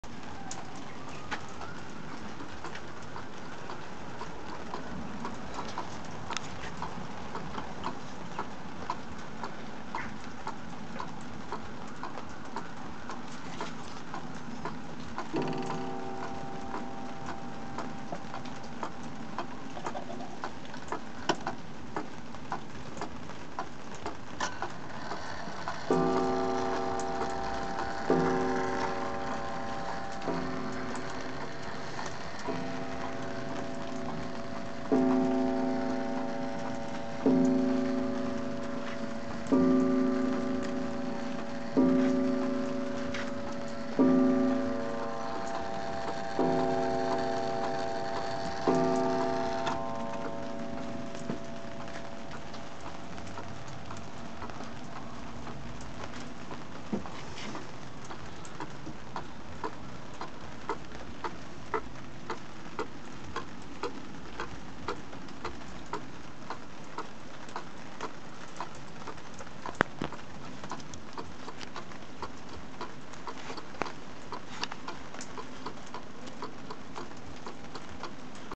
P1050810
Seth Thomas antique wall clock working with key & pendulum